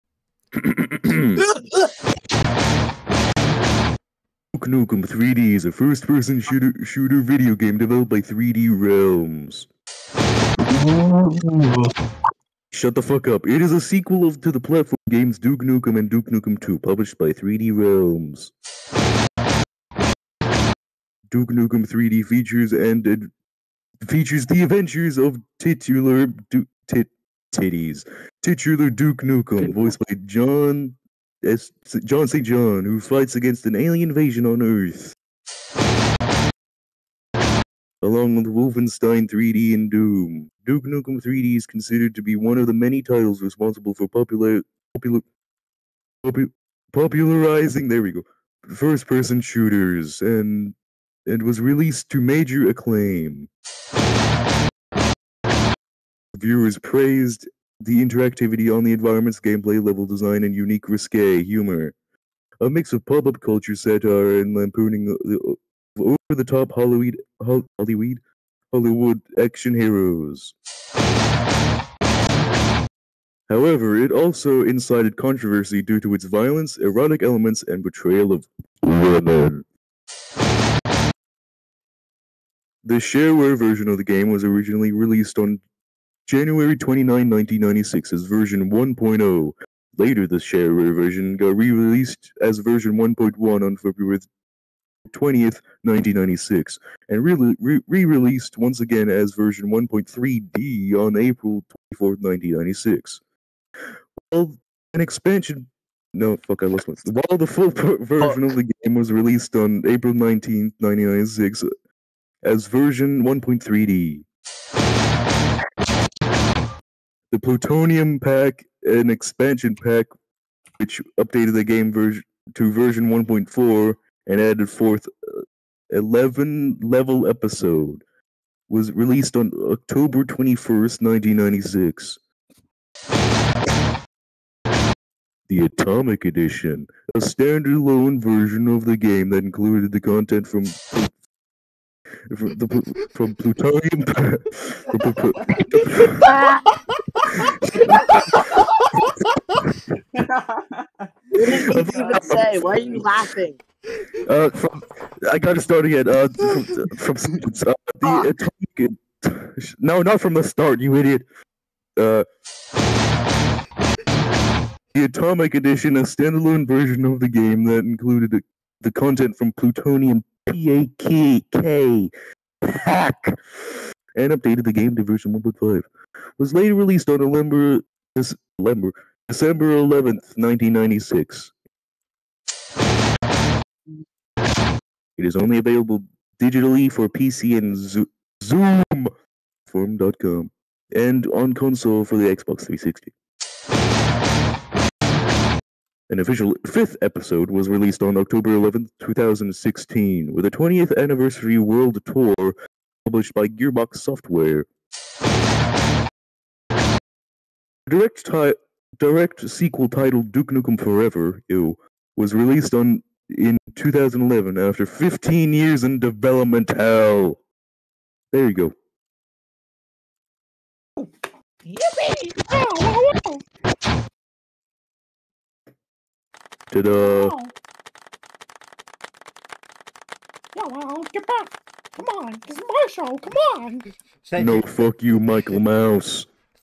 Duke Nukem Reads the Official Duke Nukem Wikipedia Article While I Draw in a Discord Call
Nuke Dukem